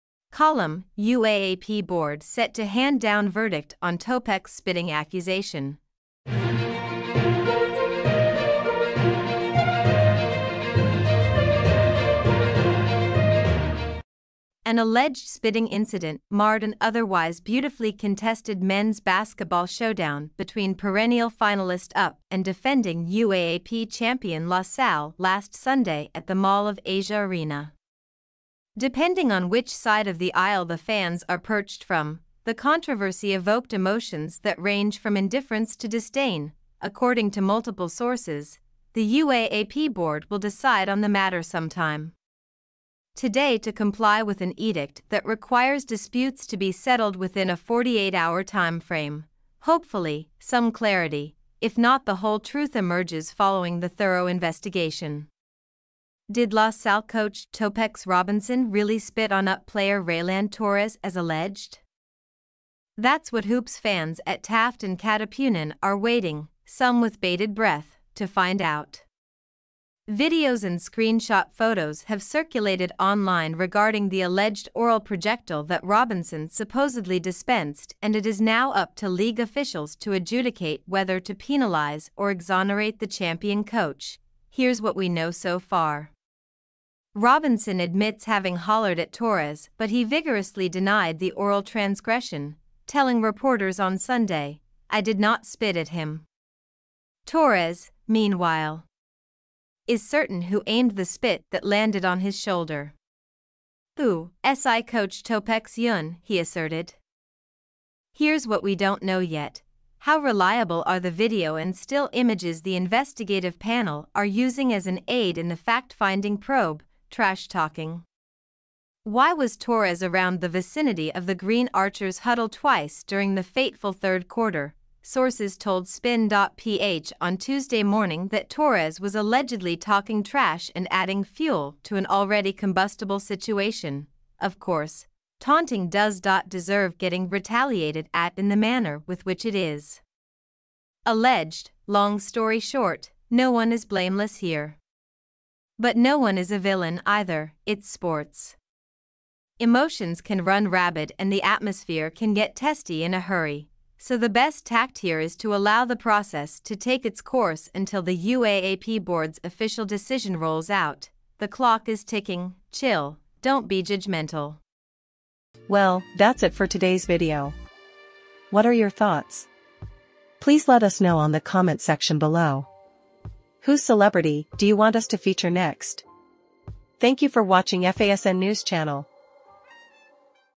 UAAP board set to hand down verdict on Topex spitting accusation
TITLE: UAAP board set to hand down verdict on Topex spitting accusation

DESCRIPTION:
An alleged spitting incident marred an otherwise beautifully contested men's basketball showdown between perennial finalist UP and defending UAAP champion La Salle last Sunday at the Mall of Asia Arena.

Depending on which side of the aisle the fans are perched from, the controversy evoked emotions that range from indifference to disdain.

According to multiple sources, the UAAP Board will decide on the matter sometime today to comply with an edict that requires disputes to be settled within a 48-hour time frame.

Hopefully, some clarity, if not the whole truth emerges following the thorough investigation.

Did La Salle coach Topex Robinson really spit on UP player Reyland Torres as alleged?

That's what hoops fans at Taft and Katipunan are waiting, some with bated breath, to find out.

Videos and screenshot photos have circulated online regarding the alleged oral projectile that Robinson supposedly dispensed and it is now up to league officials to adjudicate whether to penalize or exonerate the champion coach.

Here's what we know so far.

Robinson admits having hollered at Torres but he vigorously denied the oral transgression, telling reporters on Sunday, "I did not spit at him."

Torres, meanwhile. is certain who aimed the spit that landed on his shoulder. "Oo, si coach Topex yun," he asserted.

Here's what we don't know yet.

How reliable are the video and still images the investigative panel are using as an aid in the fact-finding probe?

TRASH TALKING?
Why was Torres around the vicinity of the Green Archers' huddle twice during the fateful third quarter?

Sources told SPIN.ph on Tuesday morning that Torres was allegedly talking trash and adding fuel to an already combustible situation.

Of course, taunting does dot deserve getting retaliated at in the manner with which it is alleged.

Long story short, no one is blameless here. But no one is a villain, either.

It's sports. Emotions can run rabid and the atmosphere can get testy in a hurry.

So the best tact here is to allow the process to take its course until the UAAP board's official decision rolls out.

Chill, don't be judgmental.